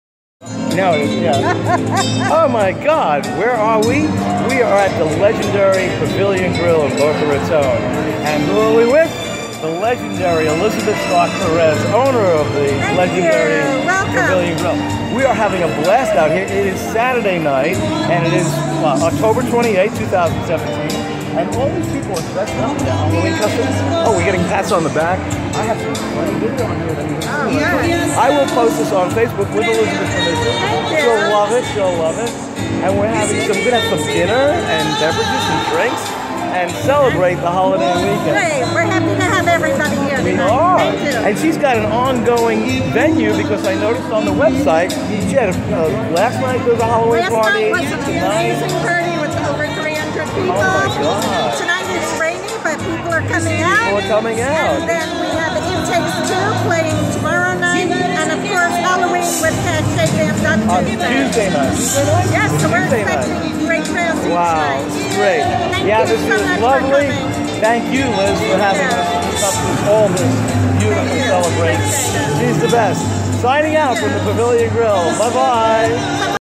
1.) Boca Pavilion Grille Halloween Party - Sat., Oct.28th, 2017
What a wildly, wonderful night out with the Ghouls & Goblins of the Pavilion Grille. Fantastic entertainment to dance & sing along to performed by the Paul Anthony Band.  Spirits were high and the table-side company was phenomenal as we ate, drank, laughed, and were more merry than scary actually!  Lol!  It was great meeting old friends and making new ones as birthday parties were also in full swing for celebration!  Aside from desserts, the diversified entrees and appetizers were absolutely scrumptious and mouth-watering! And I might add another unexpected delight of being part of a Halloween Costume Contest (and since I was part of it I did not record it on video,.. there's a limit of being too conspicuous you know,.. and even I'm aware of that,.. Lol).  And best of all, prizes were awarded for costumes judged, and I was delighted to be one of the winners!!!  Thank you Elizabeth Stark Perez for your generosity and good will to all your patrons of whom you are a dedicated servant to, and a community blessing to serve those who enter your establishment at the Pavilion Grille as you provide a fabulous ambience of family, fun, and well being!!!